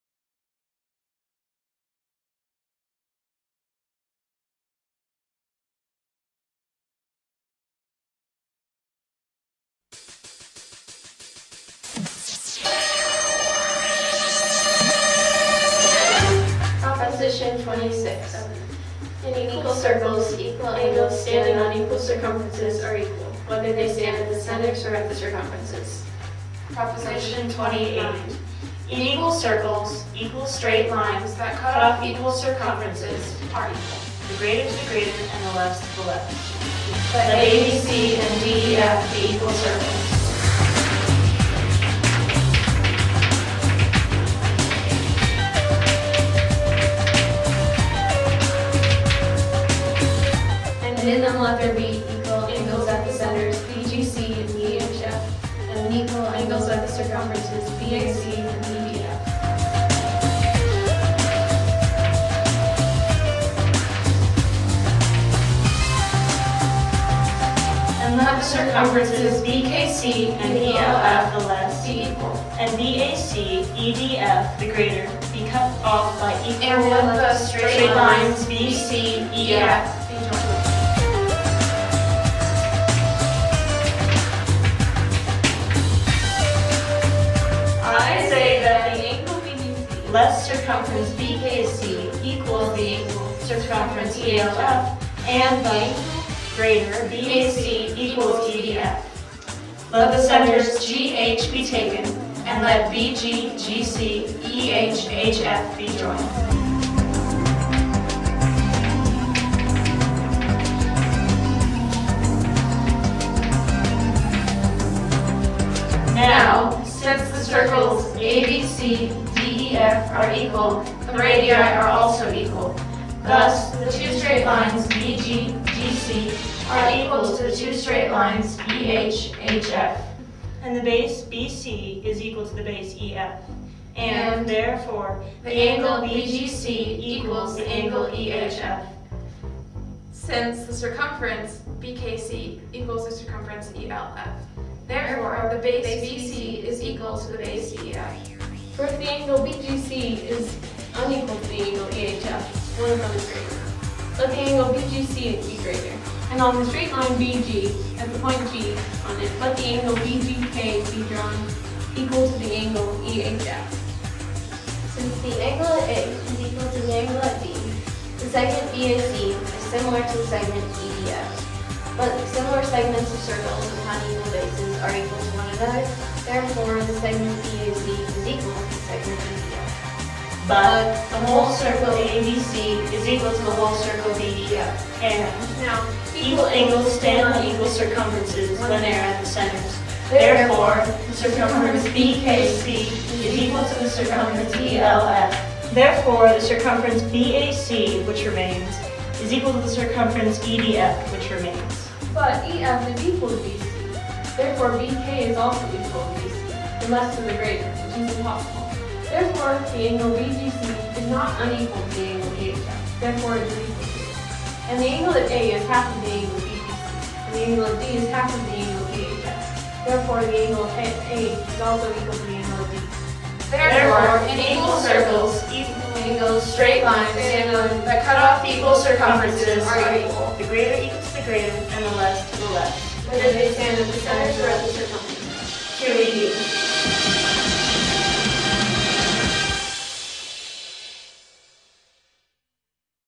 Wyoming Catholic College: Synchronized Euclid III.26-29
Synchronized Euclid: four propositions at the same time. Book III, propositions 26-29.